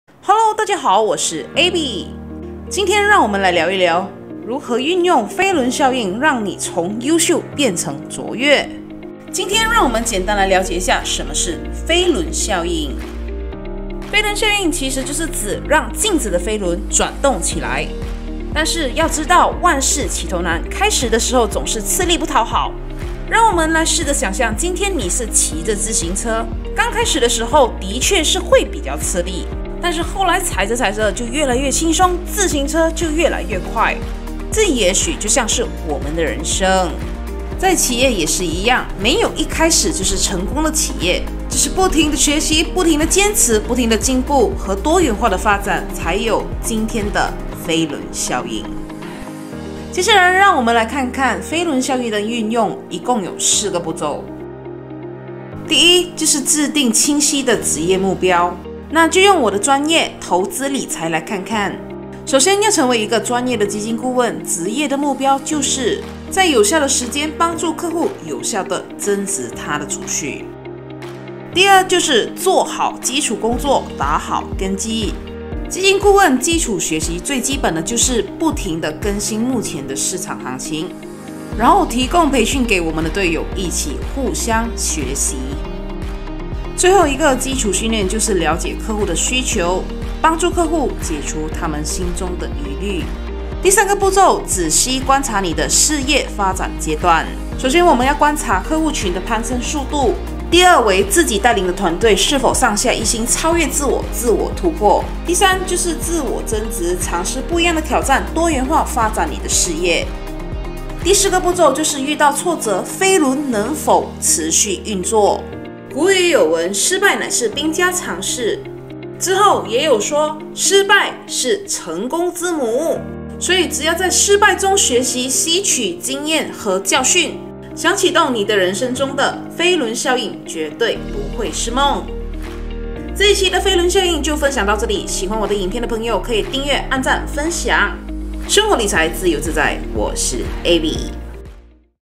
飞轮效应
今天会分享关于“飞轮效应”
如何影响你的人生，
如何让你从优秀变成卓越🤩🤩🤩